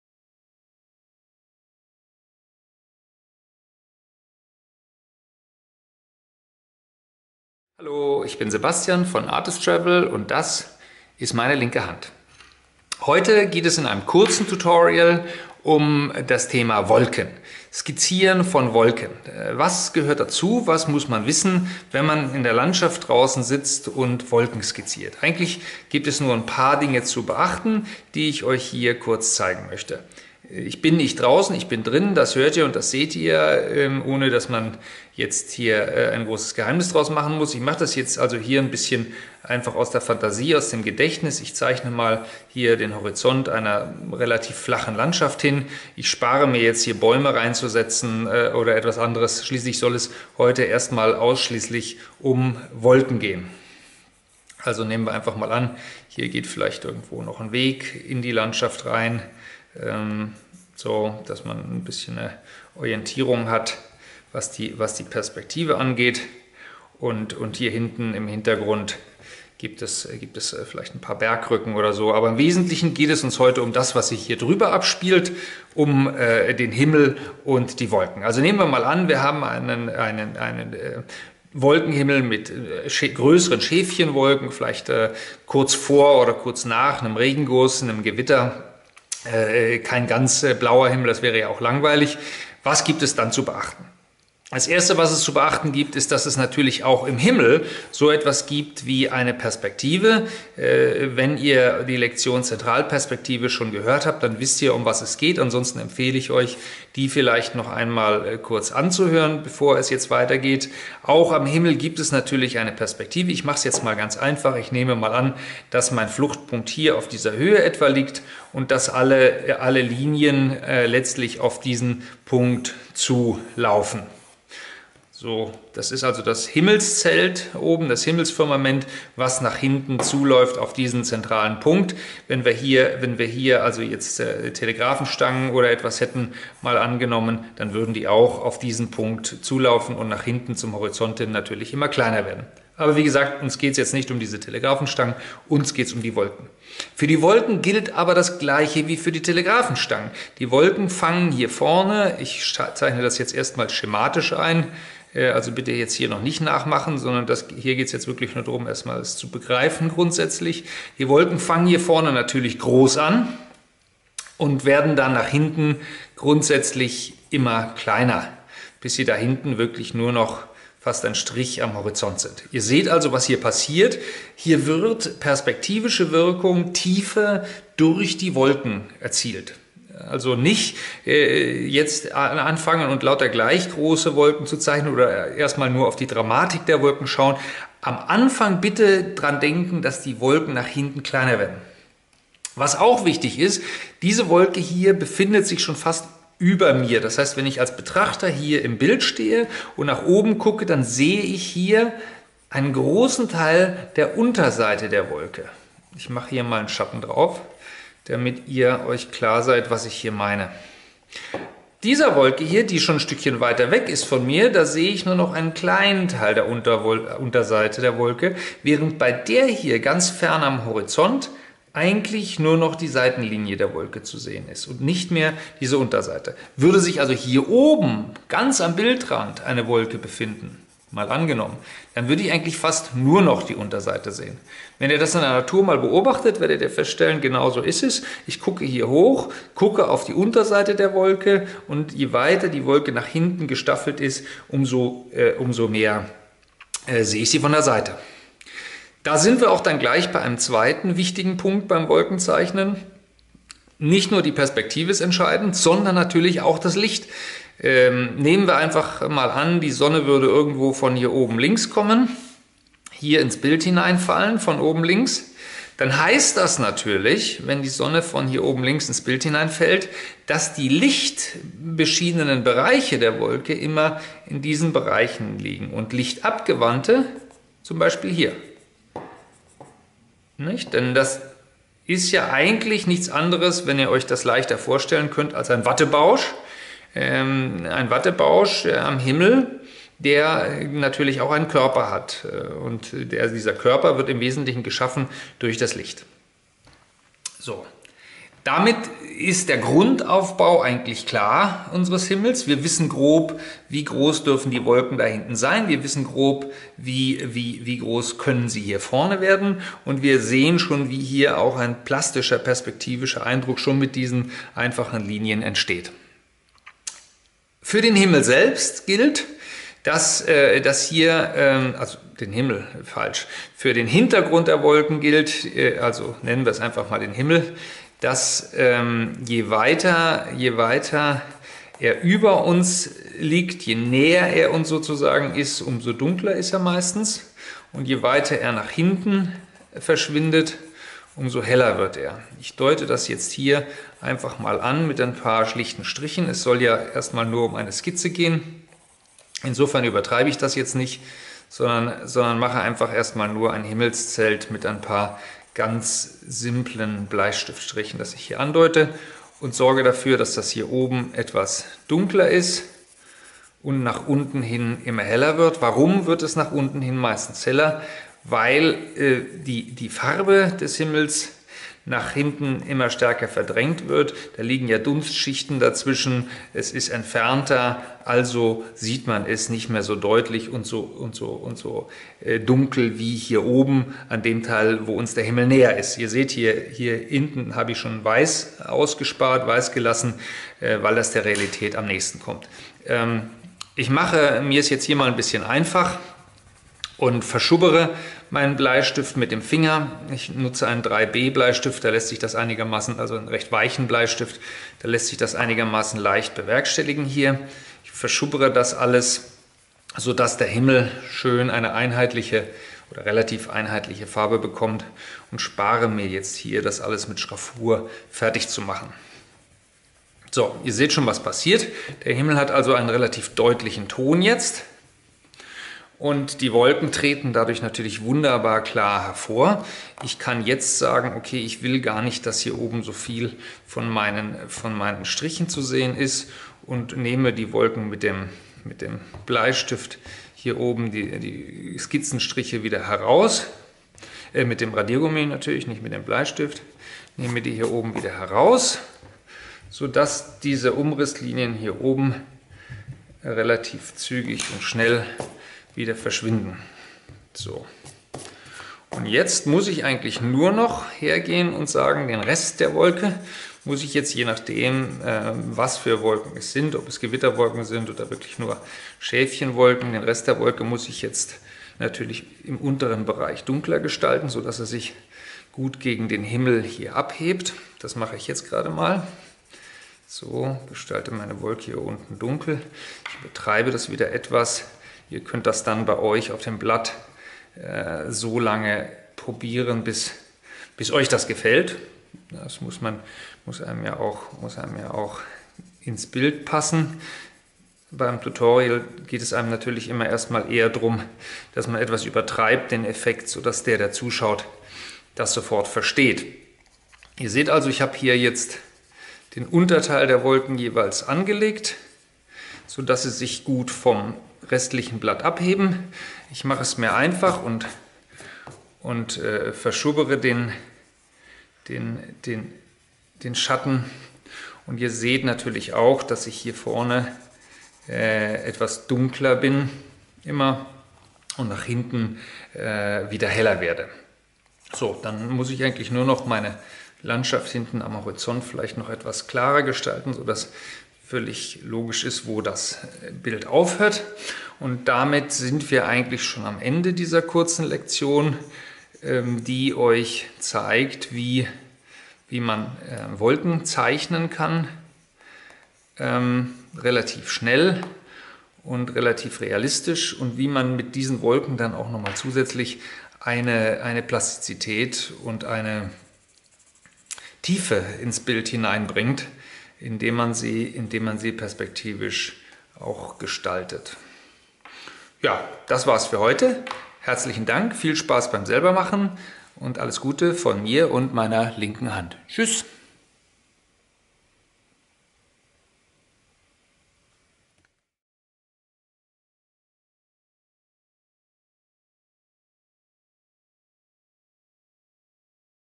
Wolken zeichnen | Ganz einfach zeichnen lernen 4
Sebastian von artistravel zeigt in diesem kurzen Video für Anfänger wie man schnell und einfach Wolken mit dem Bleistift skizziert. Wenn dir Sebastians Arbeit gefällt, dann schau dir hier seine Kurse bei artistravel an